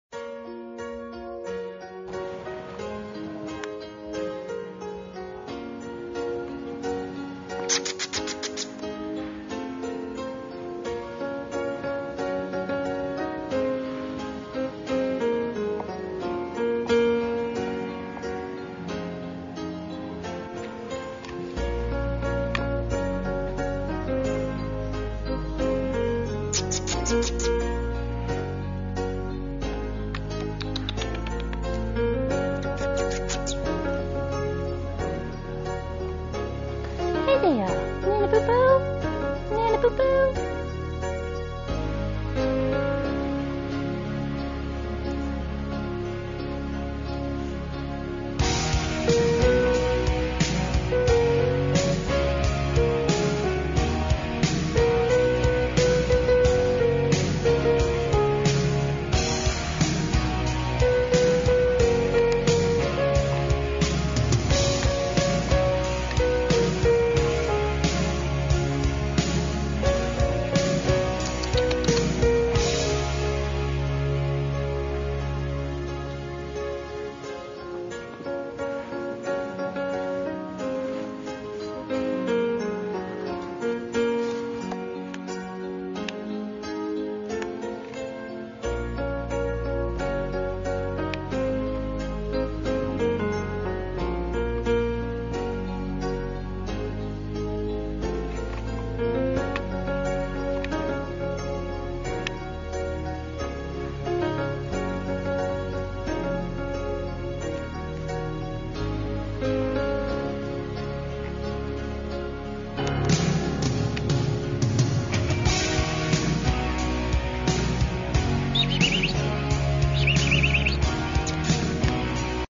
Champ! Gorgeous Tiny Tri Colored Male Puppy! Video
Video of a cute Chihuahua puppy for sale. Champ! Gorgeous Tiny Tri Colored Male Puppy! Video